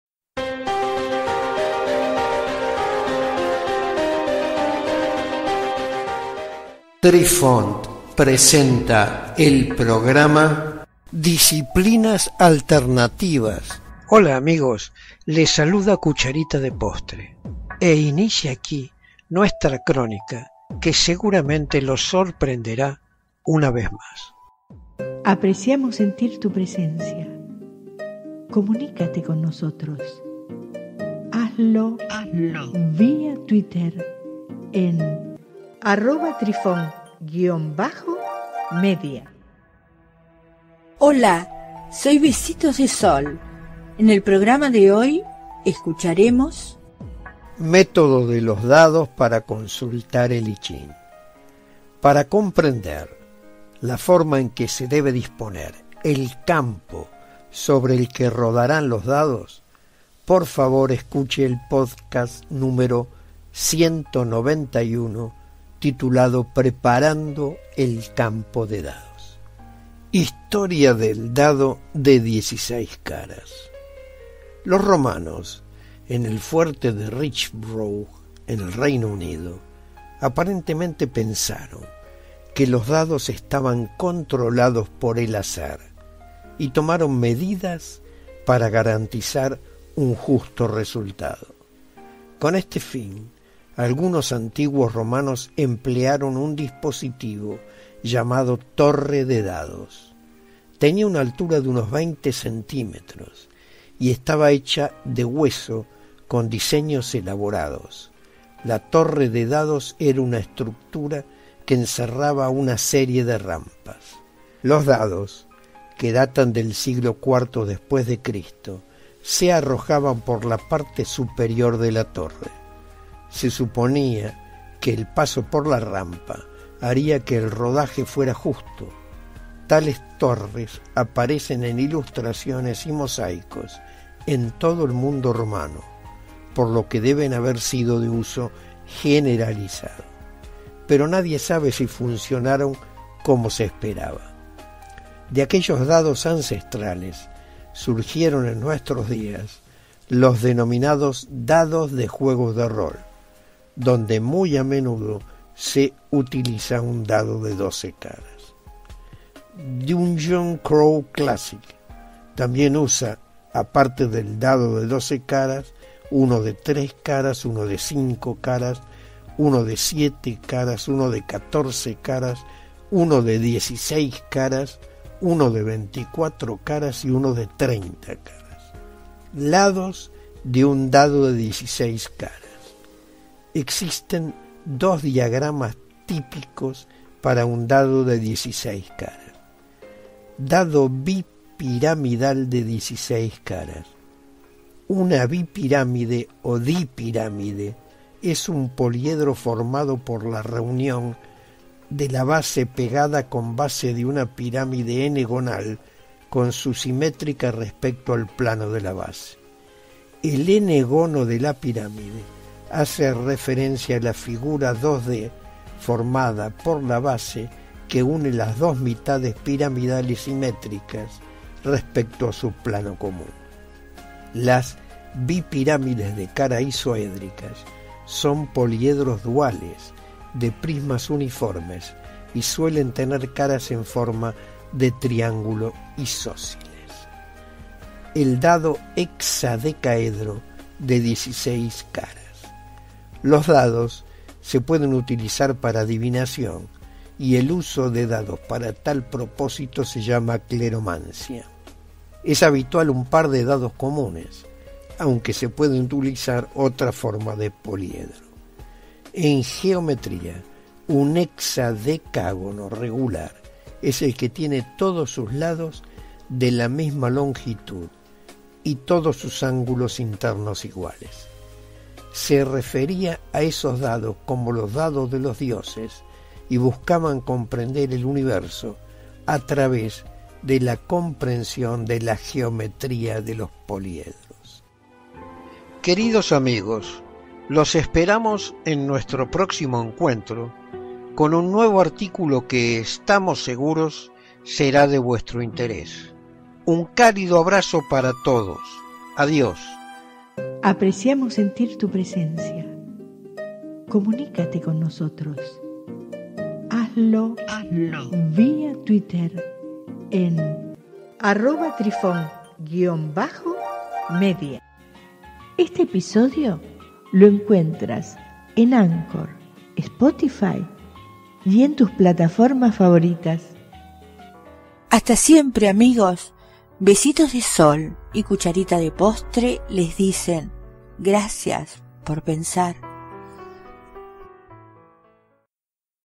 DIS-002-XVIII-200-Historia de 1d16
Los romanos en el fuerte de Richborough, en el Reino Unido, aparentemente pensaron que los dados estaban controlados por el azar y tomaron medidas para garantizar un resultado justo.
Con este fin, algunos antiguos romanos emplearon un dispositivo llamado torre de dados.
Tenía una altura de unos 20 cm. y estab hecha de hueso con diseños elaborados, la torre de dados era una estructura que encierra una serie de rampas.
Los dados, que datan del siglo IV d.C., se arrojaban por la parte superior de la torre.
Se suponía que el paso por las rampas haría que el rodaje fuera justo.
Tales torres aparecen en ilustraciones y mosaicos en todo el mundo romano, por lo que deben haber sido de uso generalizado.
Pero nadie sabe si funcionaron como se esperaba.
Oigamos el relato…